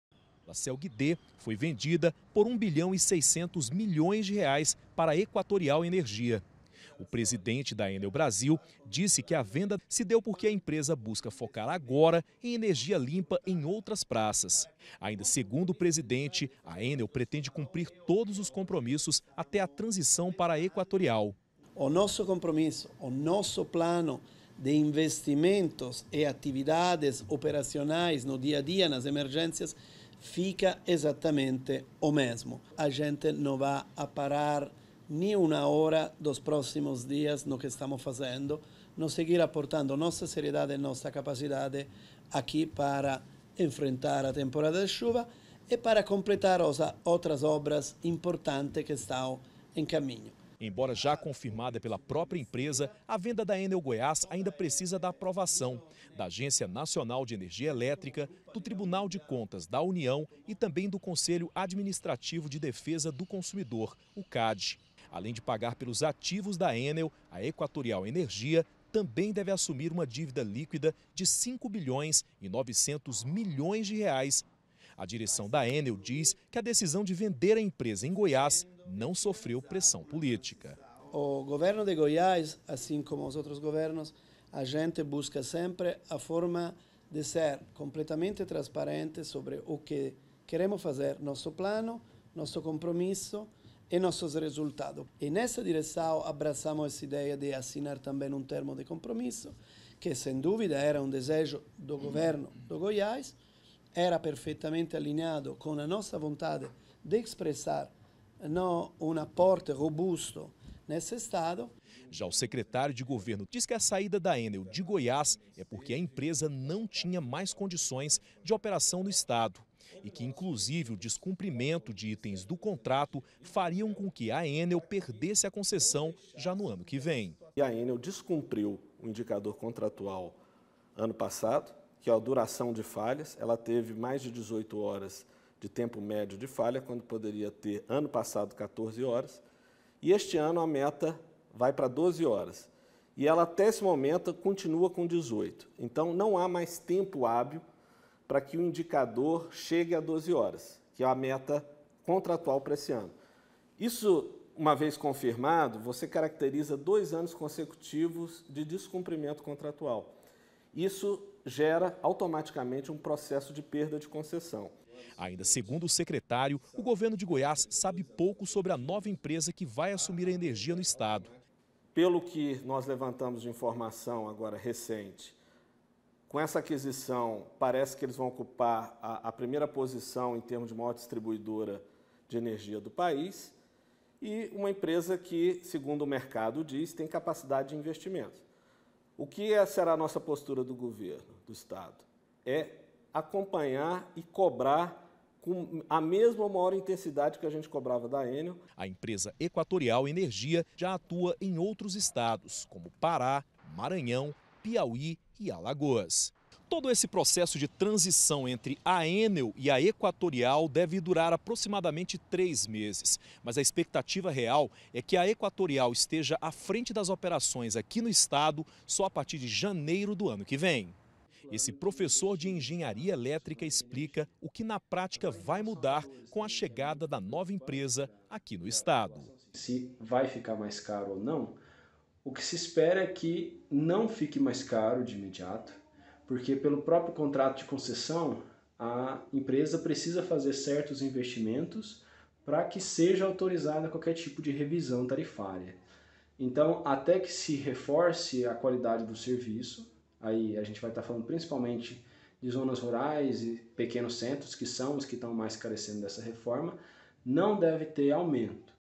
ENERGIA ELÉTRICA: O QUE MUDA COM A TROCA DA ENEL PELA EQUATORIAL
A EQUATORIAL, NOVA EMPRESA DE ENERGIA ELÉTRICA EM GOIÁS, DEVE SUBSTITUIR A ENEL A PARTIR DE JANEIRO DO ANO QUE VEM. O NEGÓCIO BILIONÁRIO FOI FECHADO NESSA SEXTA-FEIRA.